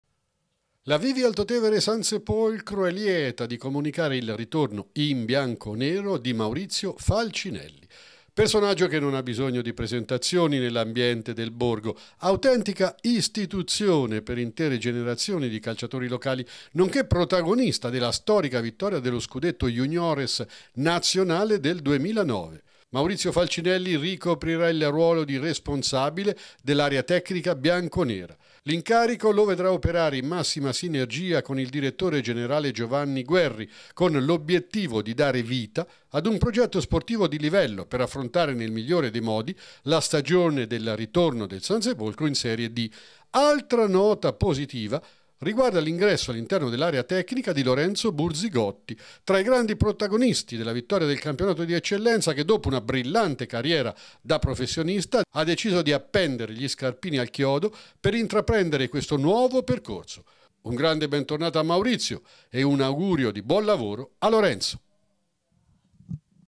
Novità nel Sansepolcro Calcio
Il ritorno di Maurizio Falcinelli e il cambio di ruolo di Lorenzo Burzigotti. Non mancano le novità in casa bianconera.